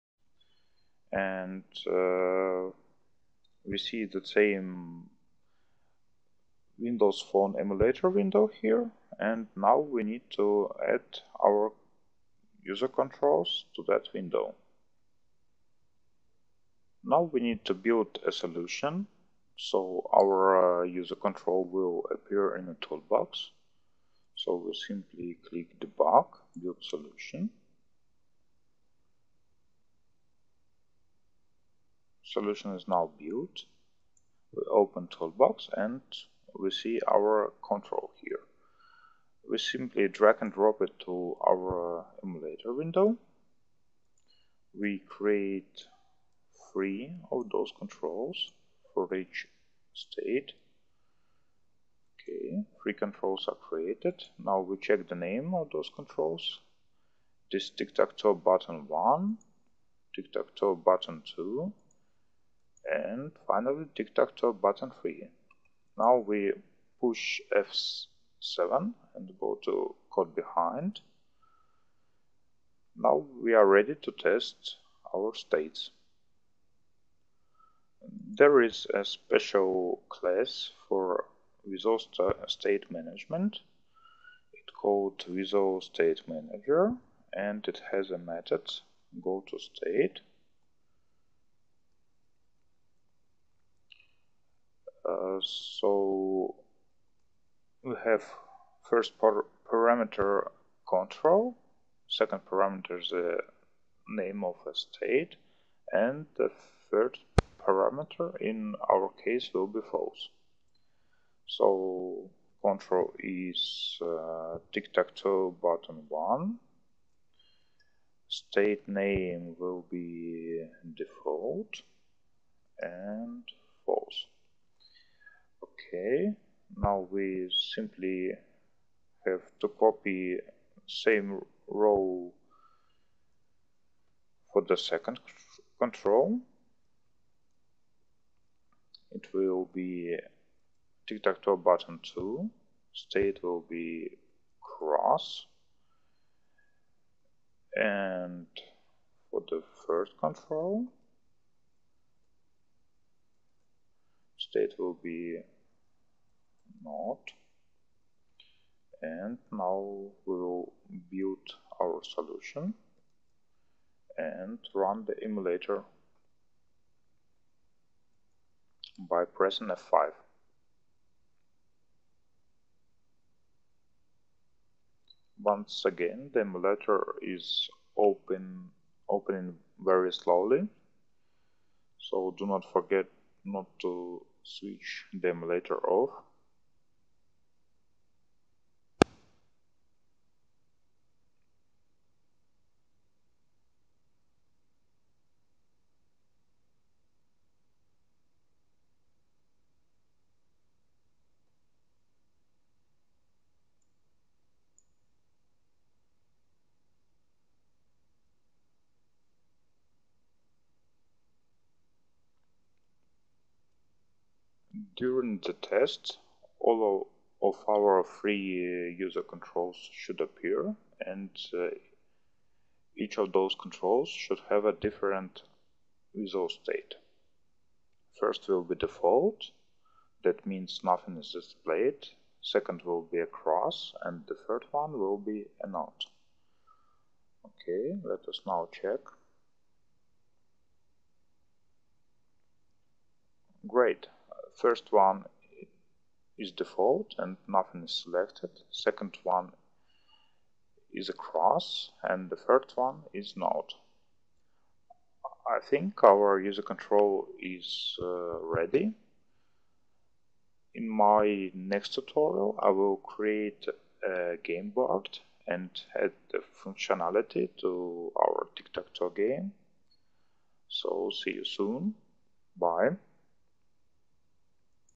Windows Phone 7 Custom Control for Tic Tac Toe by EugeneDotnet. Part 2
Part 2 of video: Creating a custom control for Tic Tac Toe game for Windows Phone 7 by EugeneDotnet.